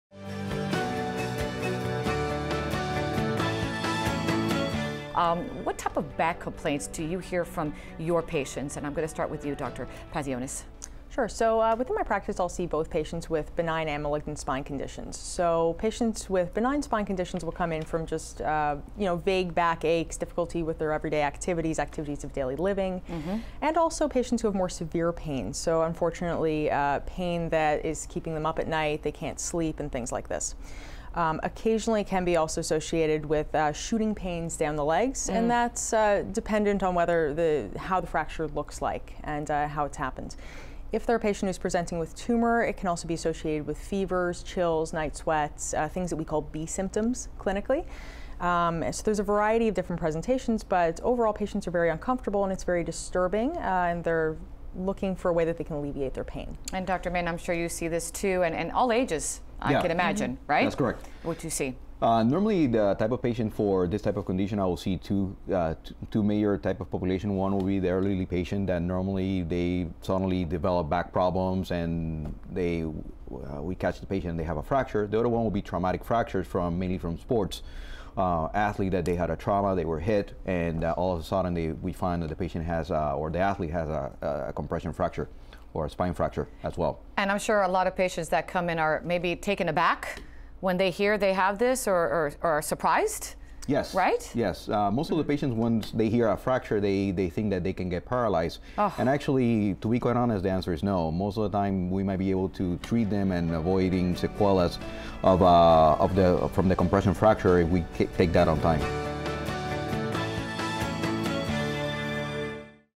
Types of Back Problems
"I'll see both patients with benign and malignant spine conditions. So, patients with benign spine conditions will come in from just vague back aches or more severe pain that is keeping them up at night," says Theresa Pazionis, Orthopedic and Spine Surgeon at Miami Cancer Institute.

According to Jose Mena, Interventional Spine Specialist at Miami Orthopedics and Sports Medicine Institute, some people will become early patients, because they suddenly develop back problems and other people can suffer from traumatic fractures, for example, from sports.